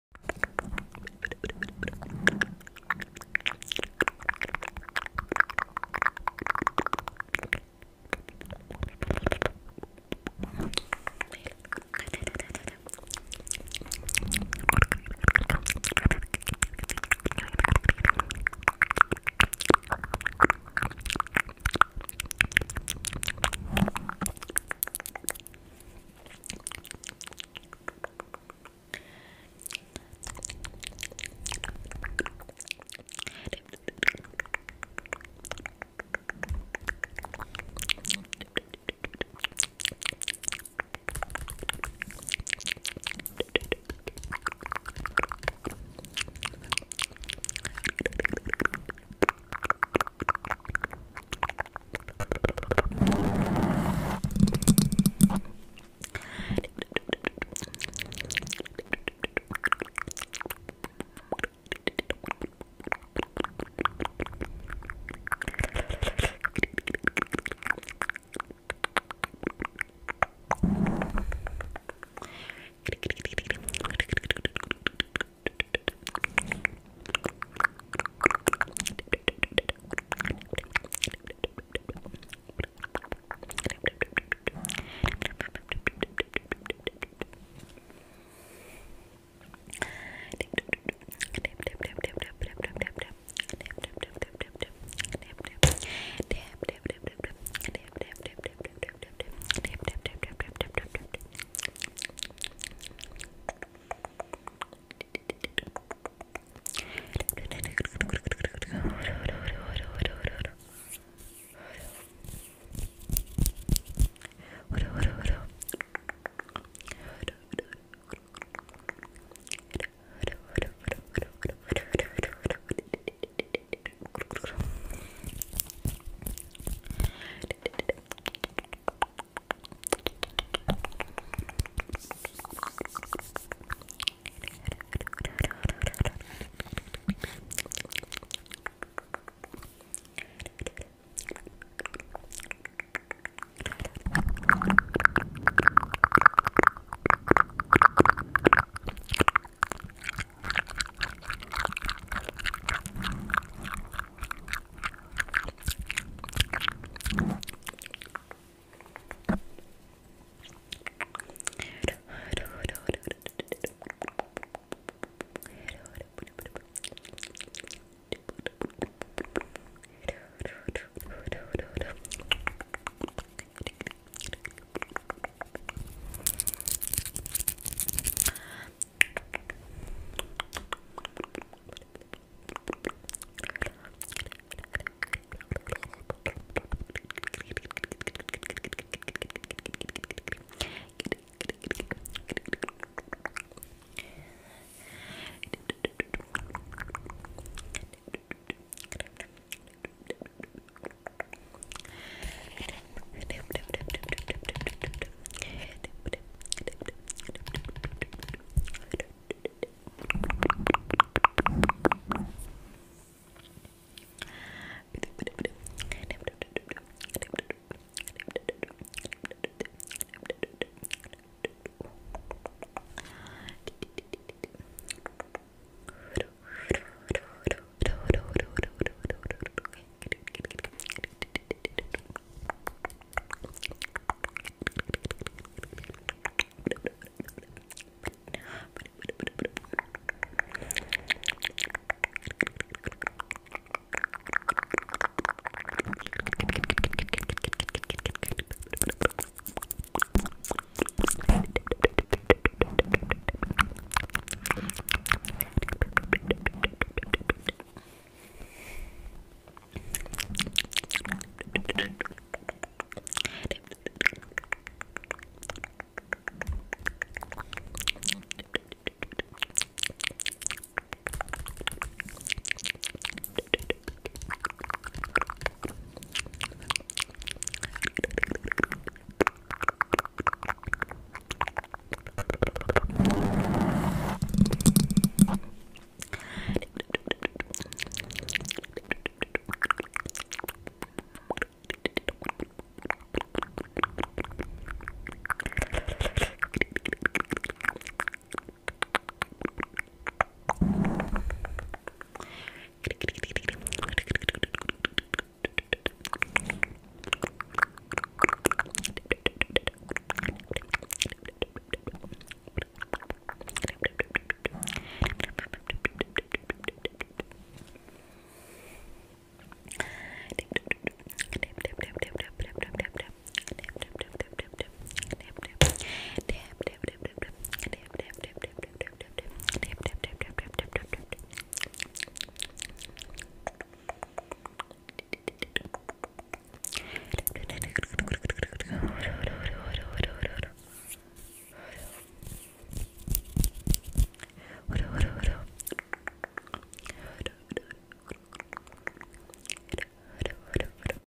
ASMR Pure Intense Mouth Sounds💙
Today i maked ASMR Pure Intense Mouth Sounds. I hope you enjoying this video & soo relaxing for you💕💕💕

Love You Guys For give me soooooo much loveeee💖💖💖💖💖💖💖💖